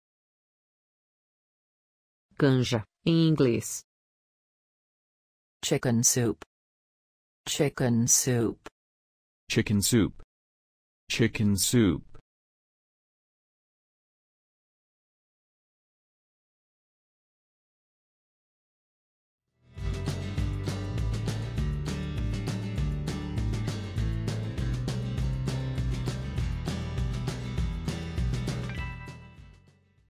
Como dizer canja em inglês
Como se diz corretamente canja em inglês? Ouça conosco.
Qual é a tradução correta da palavra canja do português para o inglês? Fale como os americanos. Além disso, como escrever corretamente a palavra chicken soup?

Obrigado!